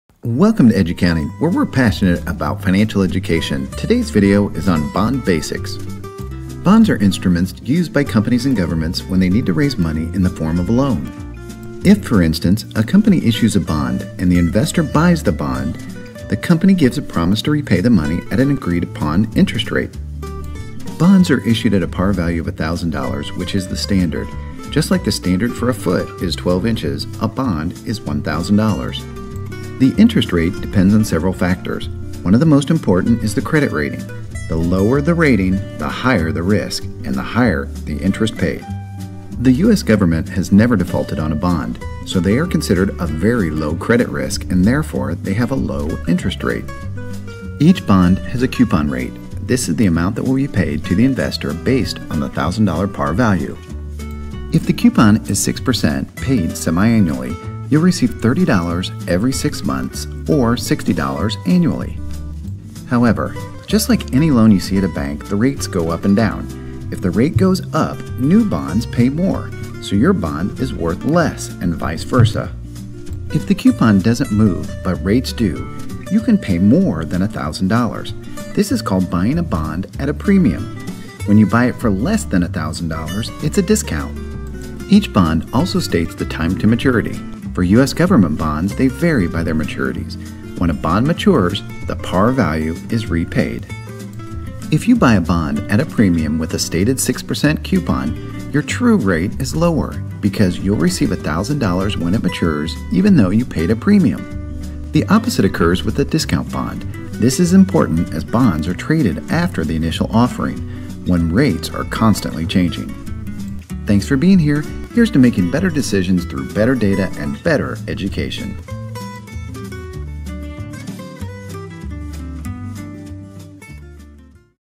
What are bonds, and what are the bond basics (Animation)?
Bonds are confusing to some. Why does their value go down, when rates go up? Check out this video to understand some of the fundamentals of bonds. They're big, and are probably in your portfolio.